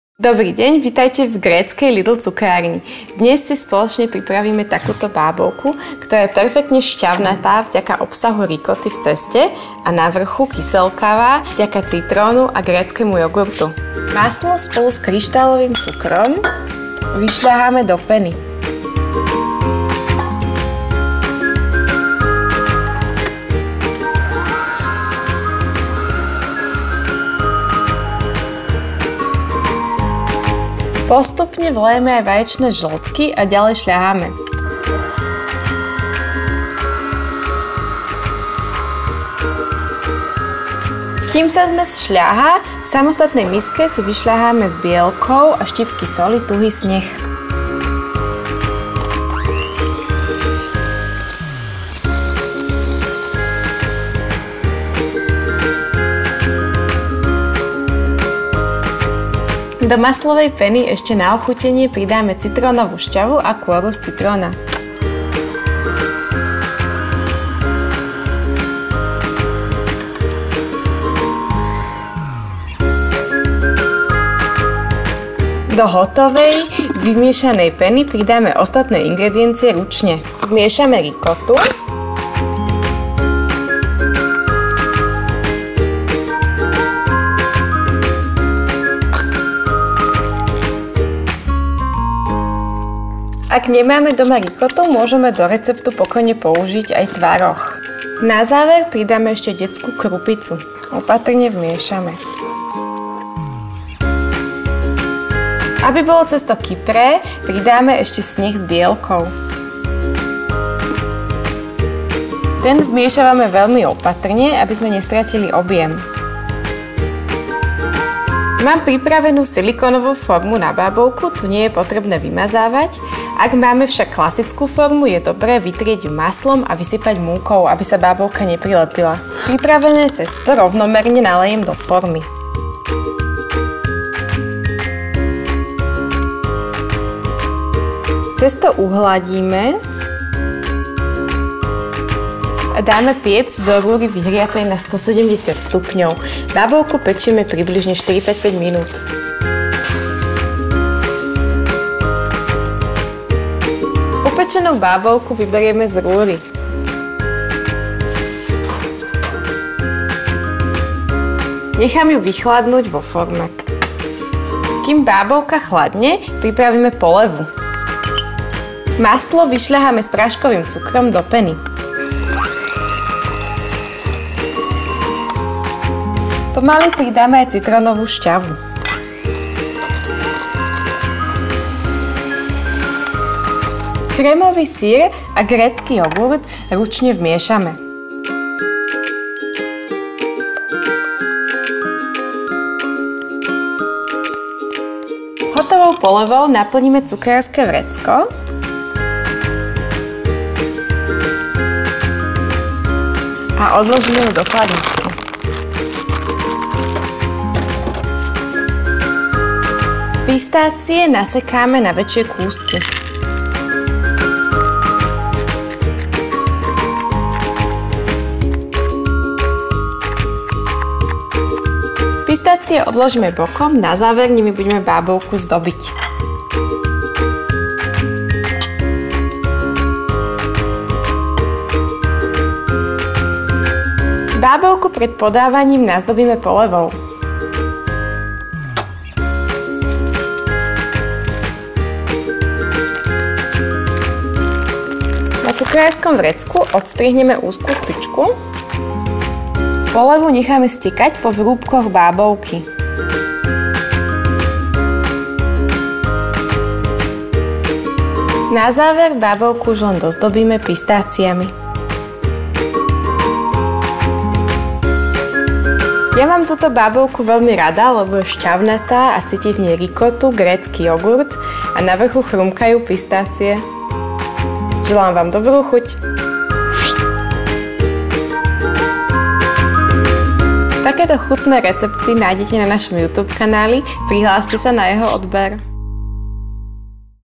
Bábovka s ricottou a citrónovou polevou | Veronika Bušová | Kuchyňa Lidla
Načerpajte skvelú inšpiráciou z LIDL Cukrárne: Bábovka s ricottou a citrónovou polevou

Obyčajné bábovky vás nudia? Veronika vás presvedčí, že nie je bábovka ako bábovka. Tú s ricottou určite nikto neodmietne. Vyskúšajte!

NÁKUPNÝ ZOZNAM

Bábovka:
•160 g zmäknutého masla Pilos
•200 g kryštálového cukru Castello
•4 vajíčka
•štipka soli Castello
•šťava a kôra z 2 citrónov
•500 g ricotty Lovilio
•200 g detskej krupice Castello

Poleva:
•60 g zmäknutého masla Pilos
•160 g práškového cukru Castello
•1 lyžica citrónovej šťavy (15 ml)
•100 g nátierky z čerstvého syra Goldessa classic
•3 lyžice gréckeho jogurtu Eridanous

Na ozdobenie:
•80 g nesolených pistácií Alesto